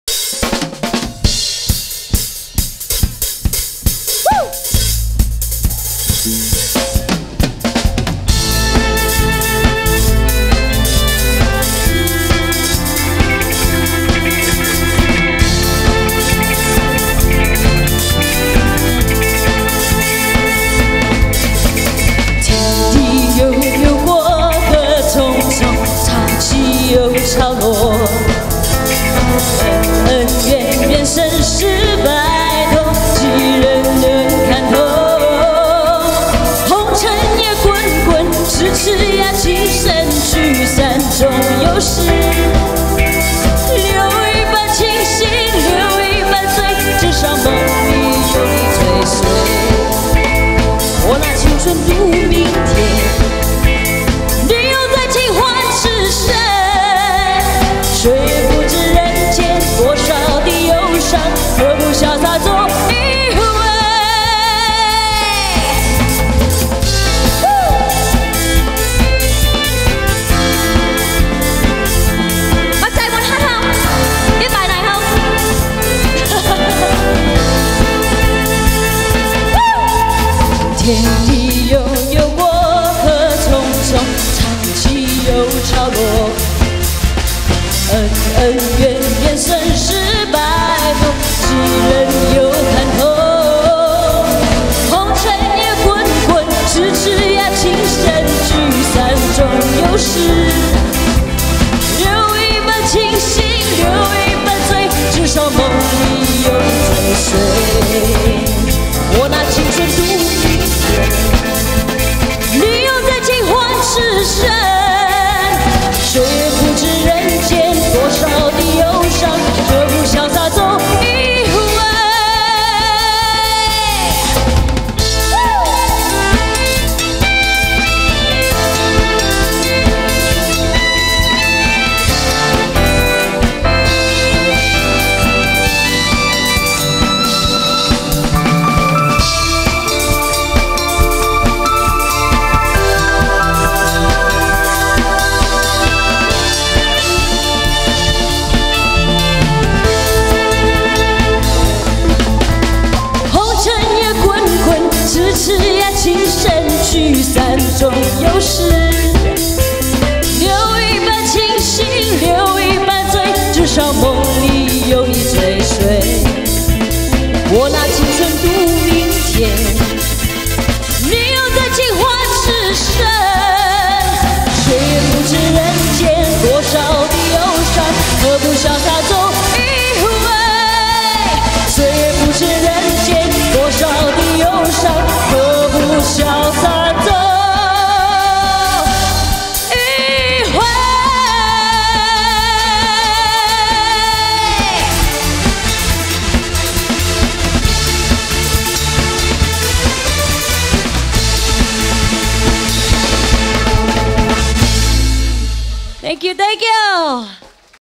Chinese Song - Linda Chou & The Night Band
Live Recording!
60th Wedding Anniversary on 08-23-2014